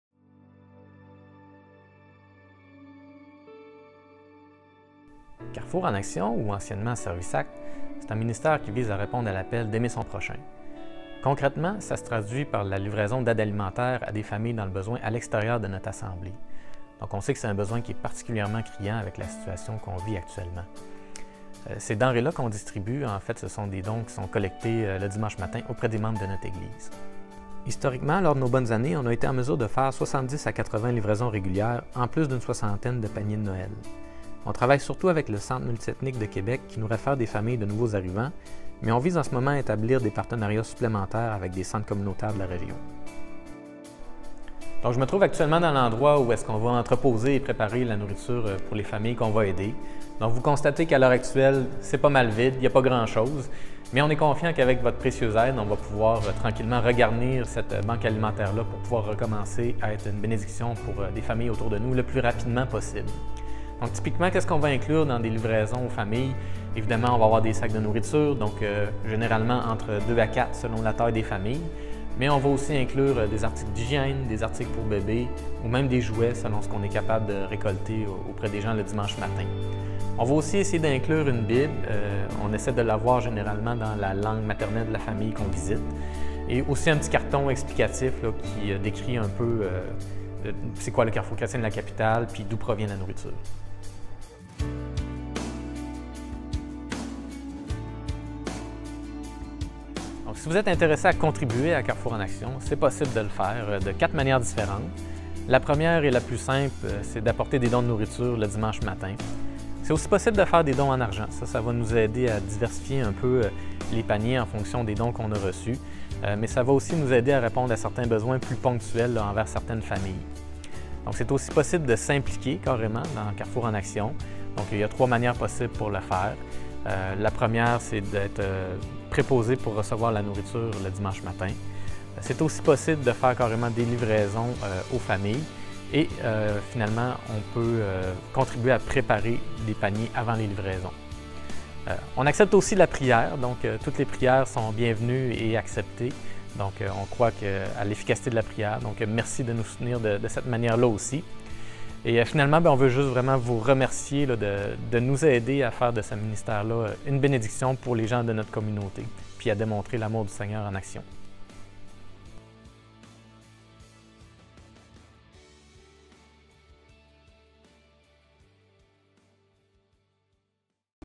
Présentation de Carrefour en action
Lancement du ministère Carrefour en action
Olivier Marcotte
27 mars 2022